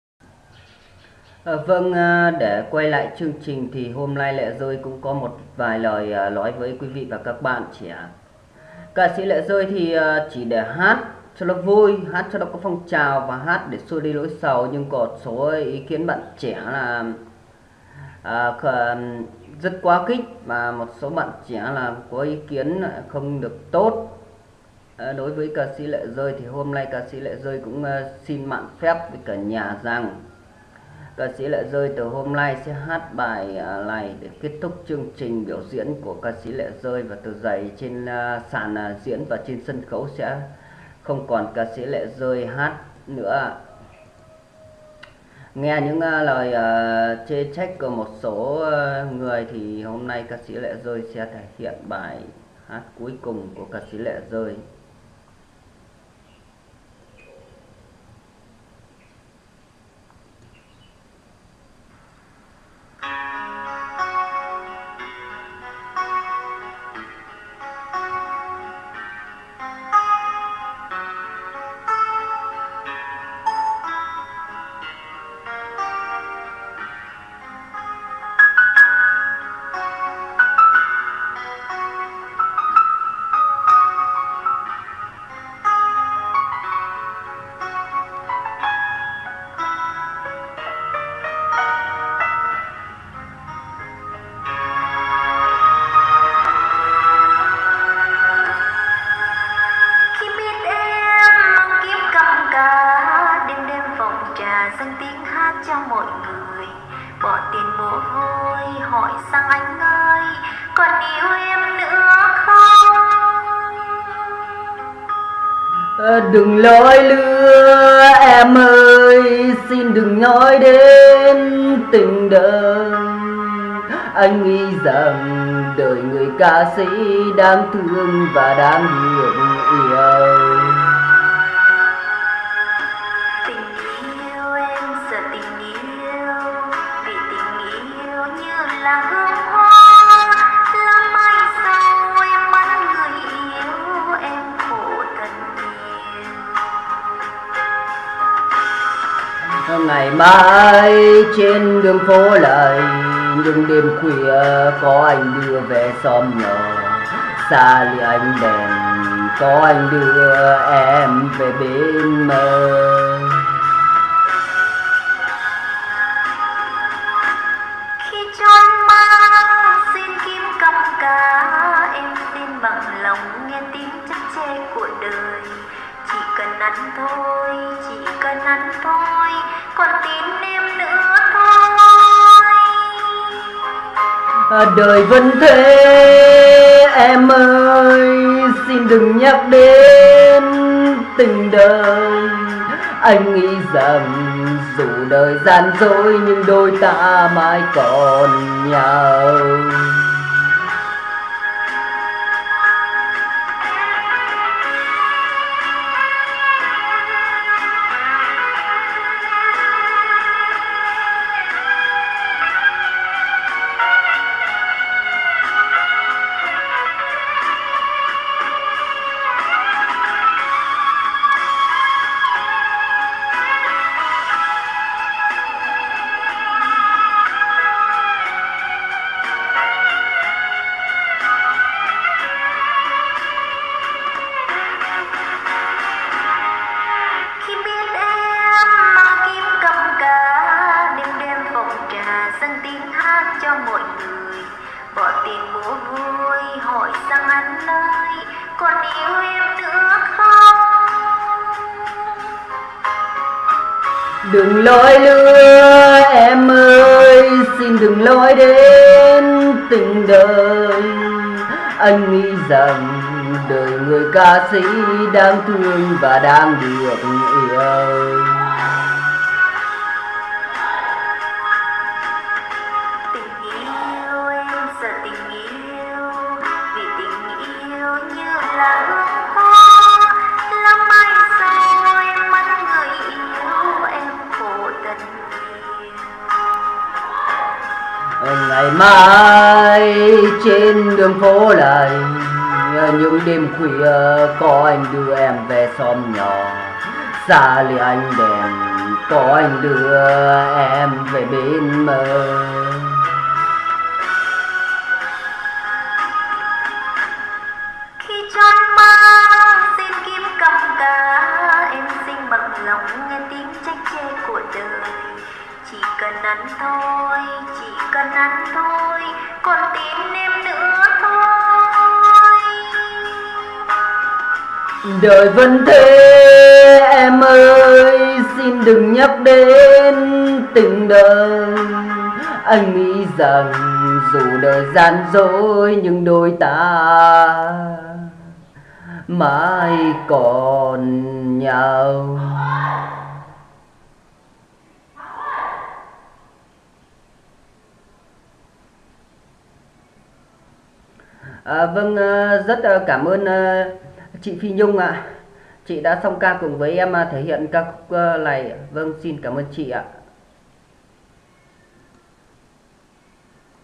Ca sĩ Lệ Rơi chính thức giải nghệ hát bài cuối rất cảm động
Lệ Rơi hát ca khúc cuối chia tay fan cực kỳ cảm động.

- hân hạnh mang đến cho bạn chương trình này !